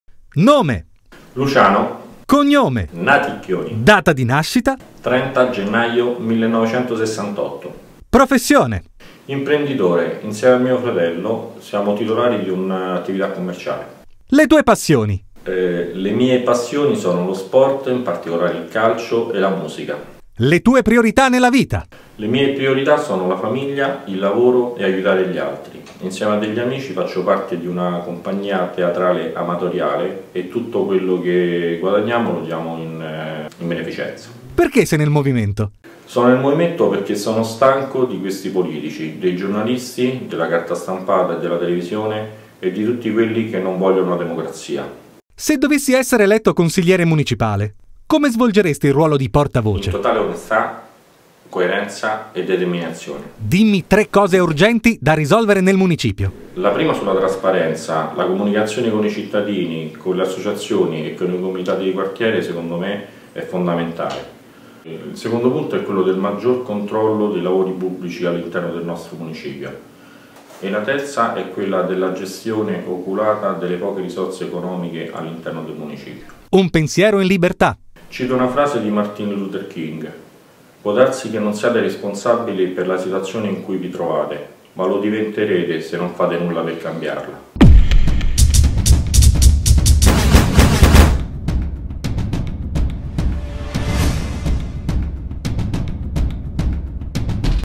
Luciano Naticchioni - Candidato portavoce consigliere municipale Roma Montemario
Conosciamo Luciano Naticchioni - Candidato portavoce consigliere municipale Roma Montemario